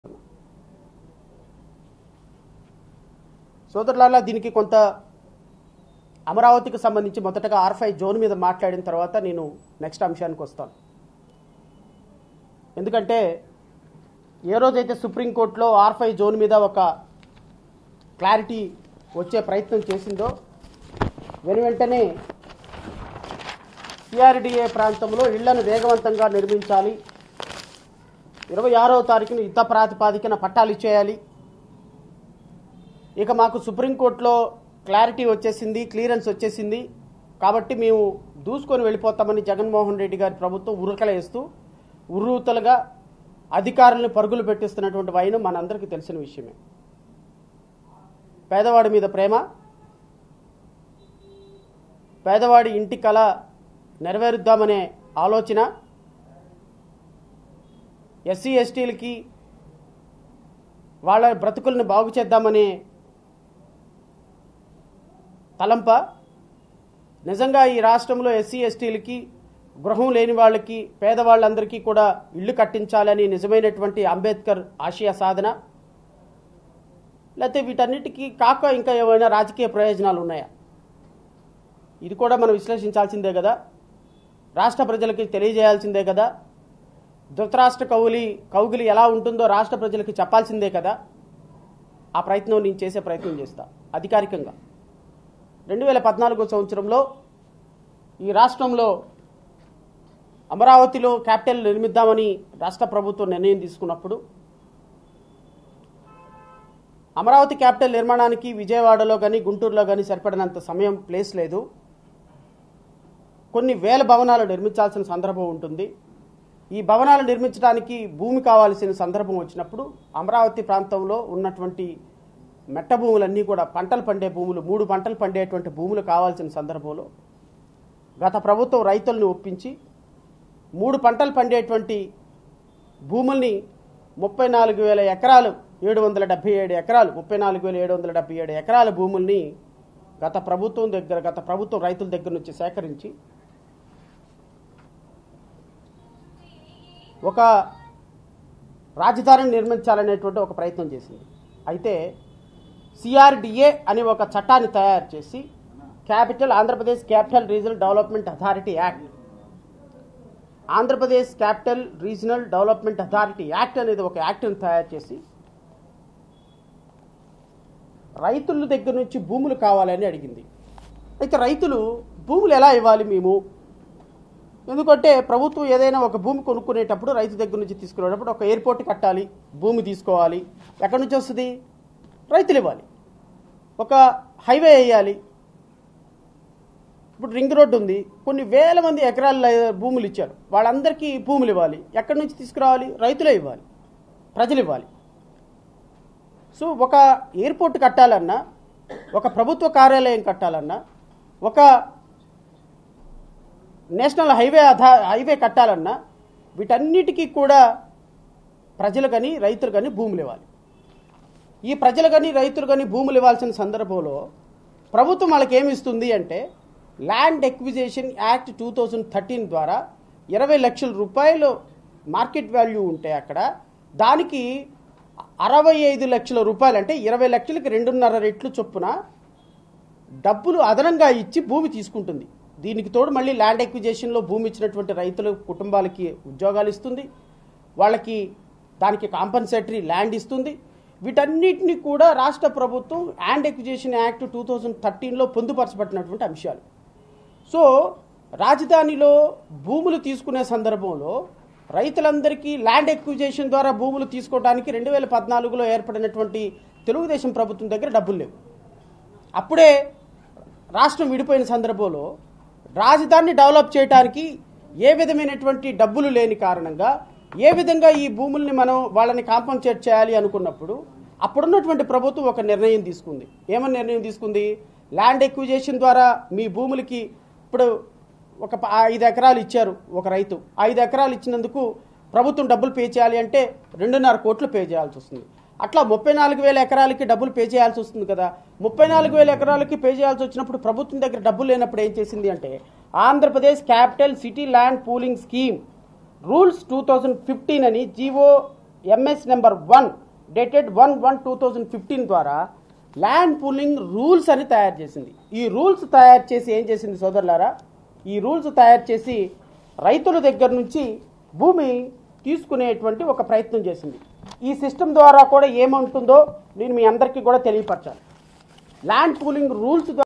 R5 జోన్ ఇష్యూ పై ఉరకలేస్తున్న జగన్! | Advocate Sravan Kumar | Amaravati R5 Zone | CM Jagan | TV5
LIVE:  R5 జోన్ పై Advocates Sravan Kumar Press Meet | CM YS Jagan | TV5 News Digital